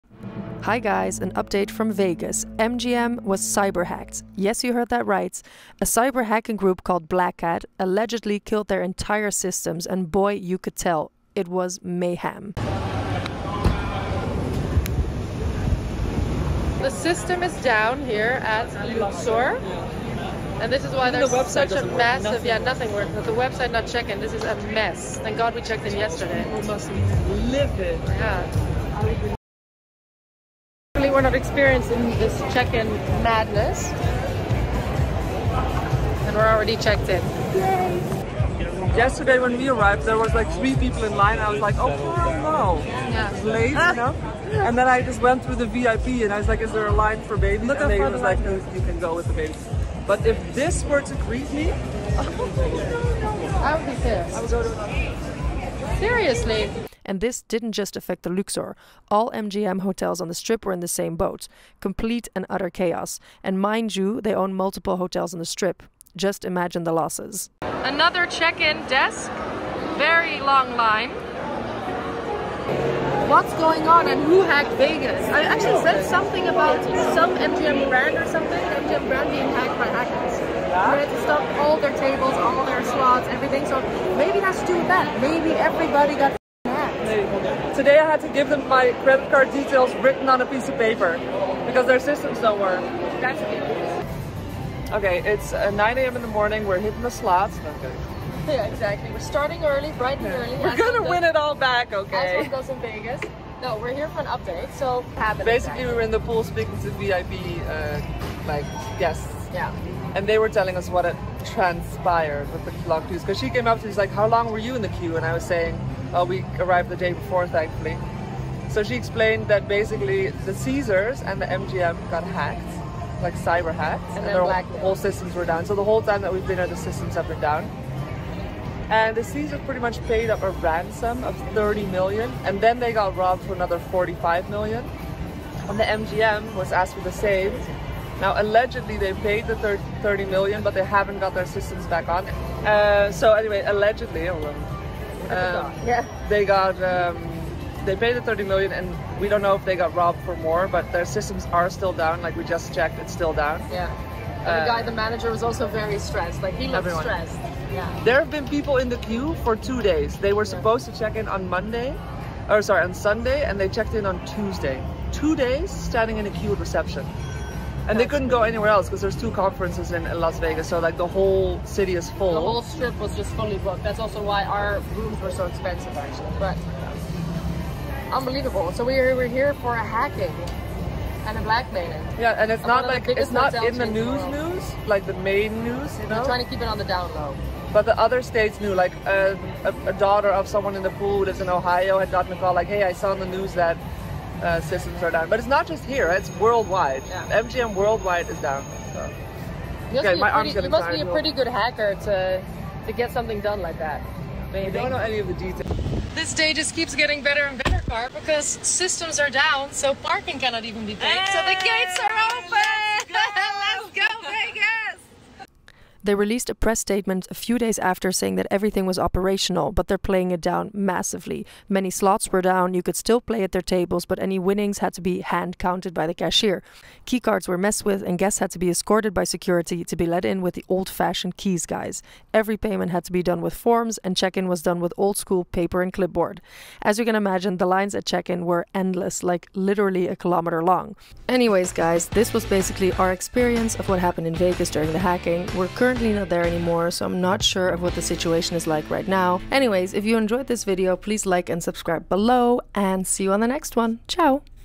MGM hacked by cyber hackers 2023 - Las Vegas
Here is my experience of the MGM hacking in Vegas when it happened. The footage is mine and shows you what the situation was like, I also got information from some vip guests at Luxor, as they were informed by staff at the hotel as to what was going on.